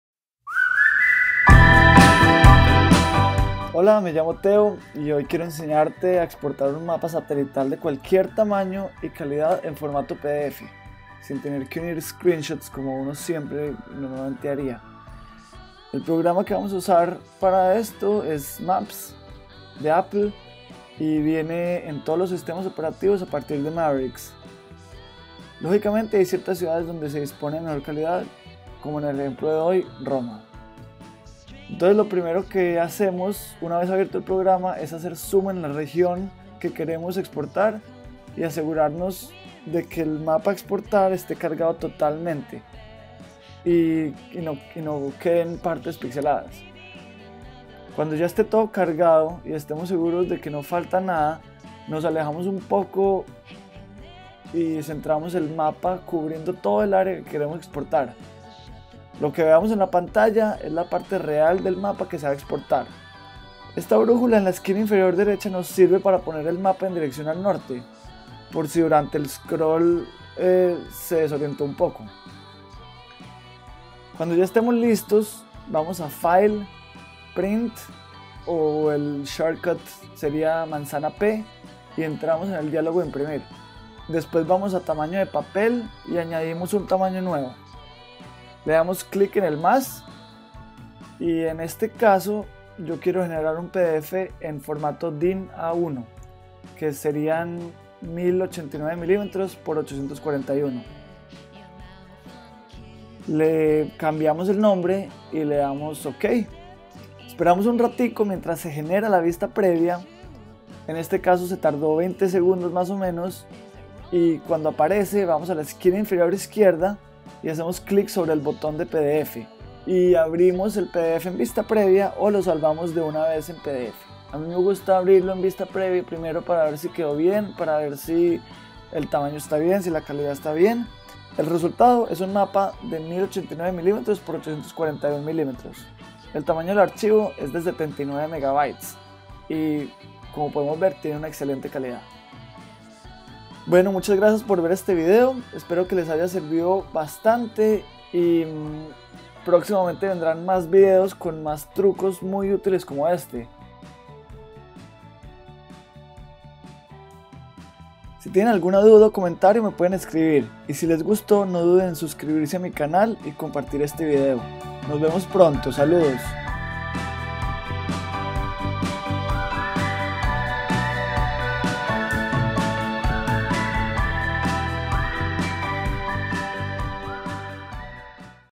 Como exportar un mapa de gran tamaño en PDF - Tutoriales Teo
En este video aprenderas a exportar un mapa, como antiguamente se hacia con google maps, pero ahora mucho mas sencillo con Maps de Apple.
Mac OSX Mavericks.

Si tienes alguna pregunta, comentario o sugerencia no dudes en escribirme. Saludos!